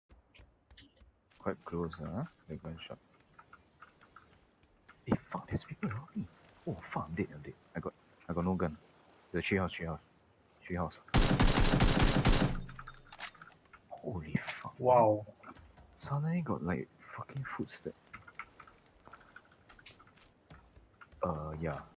Holy fuck
" Eh fuck there is people around me. Oh fuck i'm dead i'm dead. I got no gun.. Holy fuck"
- Xephna 211017